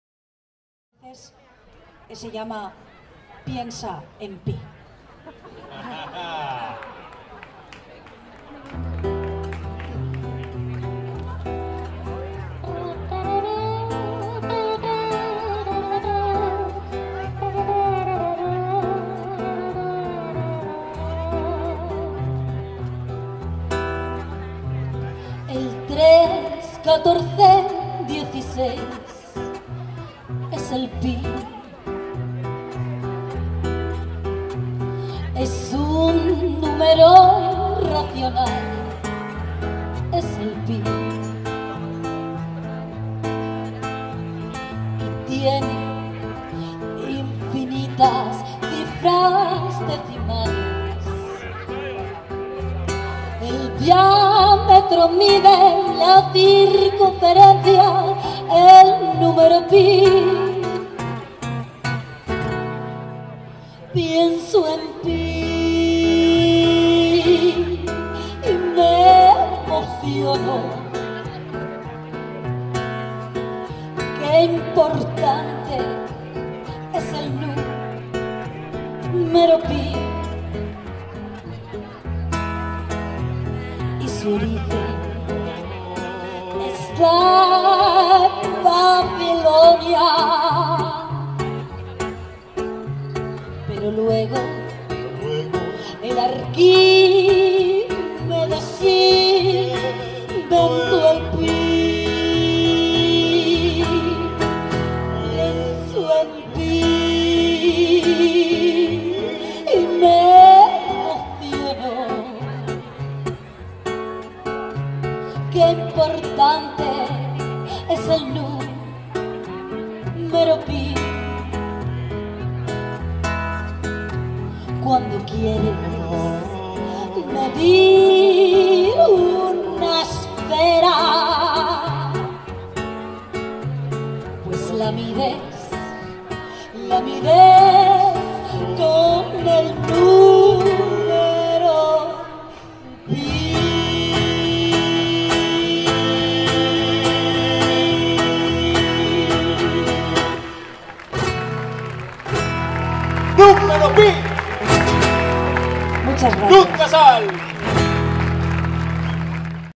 Marisol Aznar y David Angulo - Caixaforum zaragoza
Actuación de Marisol Aznar y David Angulo en la Noche Europea de los Investigadores en Caixaforum Zaragoza el día 27 de septiembre de 2019.